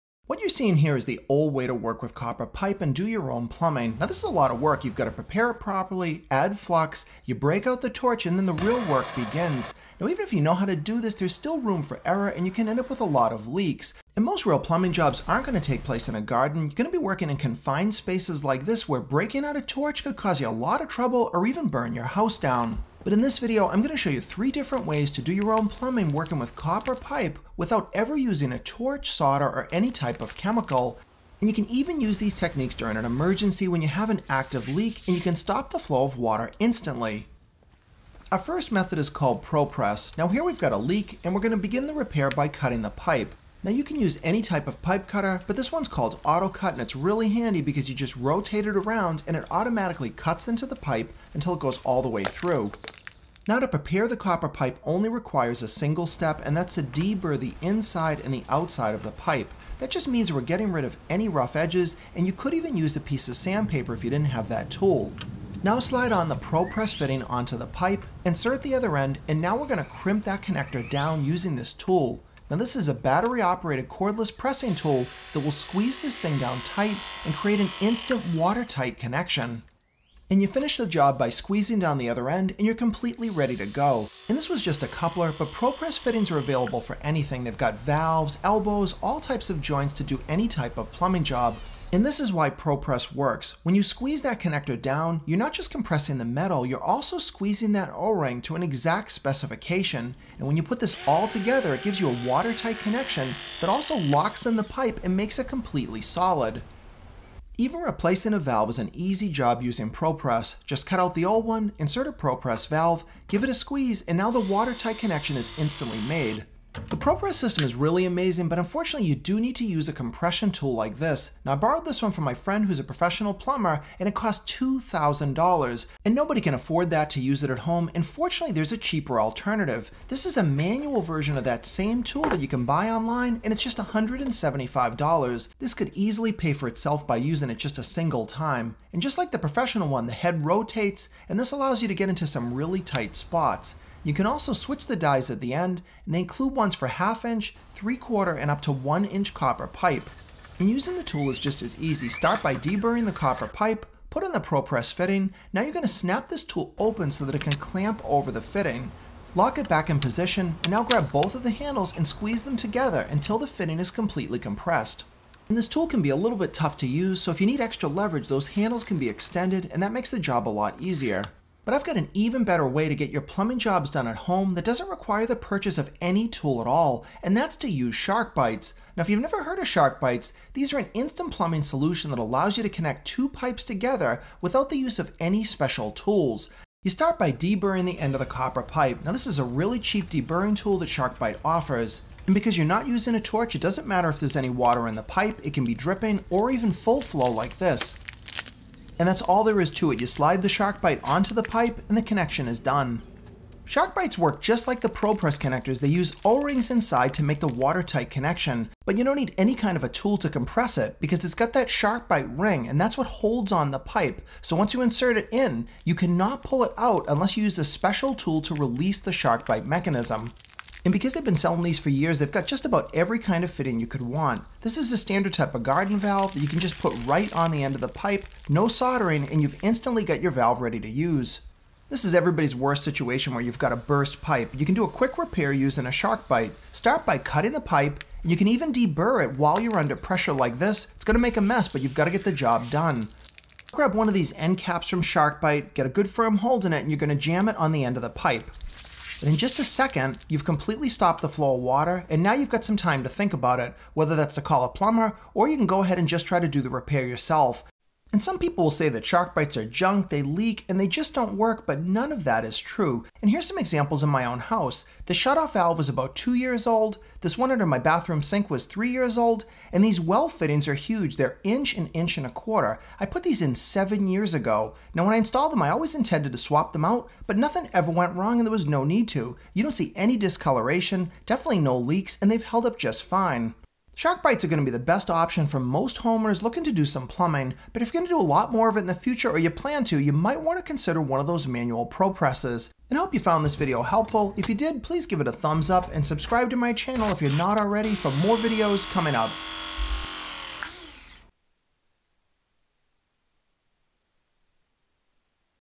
NEVER SOLDER PIPE AGAIN - 3 Ways for EASY DIY Plumbing
Plumbing has never been EASIER with 3 EASY instant ways to work with copper pipe, replace valves & make repairs with NO TORCH  NO SOLDER. I show you the LEAST expensive way through how PROS have pros have stopped using SOLDER in favor of PROPRESS & SHARKBITES & why

Manual Propress Tool
Autocut Pipe Cutter
Rigid Deburring Tool
Sharkbite Deburring Tool
Rigid Proress Tool